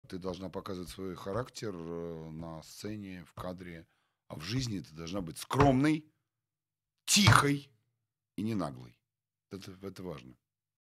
Ты должна показывать свой характер на сцене и в кадре
Новый подкаст-интервью с Михаилом Полицеймако.

У нас в гостях на подсъеме российский телеведущий, актёр театра и кино, за плечами которого более 70 ролей и участие в таких проектах, как "День радио", "День выборов", "Моя прекрасная няня". Талантливый певец, да и просто человек с прекрасным чувством юмора. Сын актрисы Марии Полицеймако и актёра Семёна Фарады.

С Михаилом у нас сложился интересный диалог не только касательно театра и профессиональных навыков. Мы поговорили про ответственность, новое поколение молодежи и раскрыли Михаила совершенно с другой стороны ;)